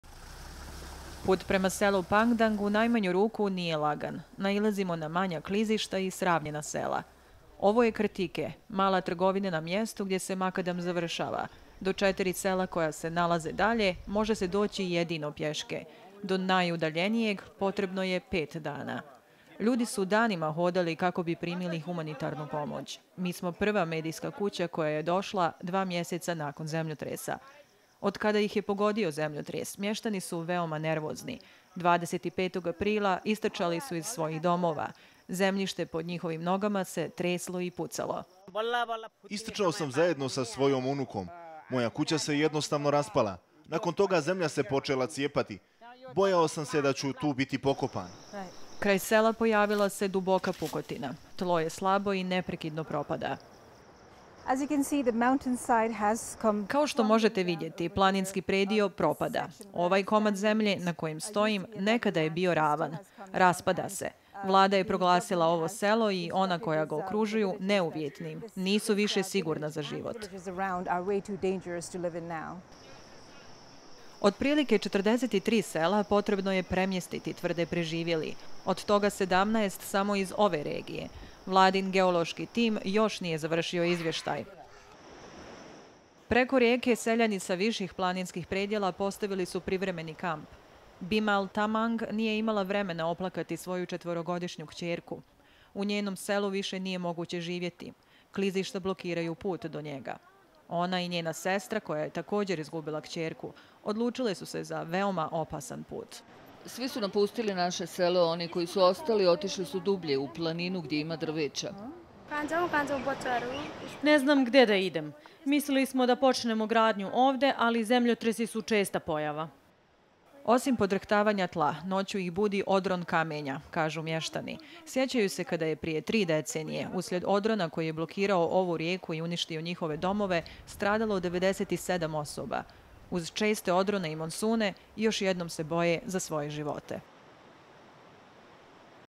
Mještani nepalskih sela u strahu od odrona
Hiljade Nepalaca u opasnosti su zbog čestih odrona nakon zemljotresa u aprilu koji je znatno oslabio planinske dijelove.
Traži se premještanje sela. Ali, konkretnog plana nema.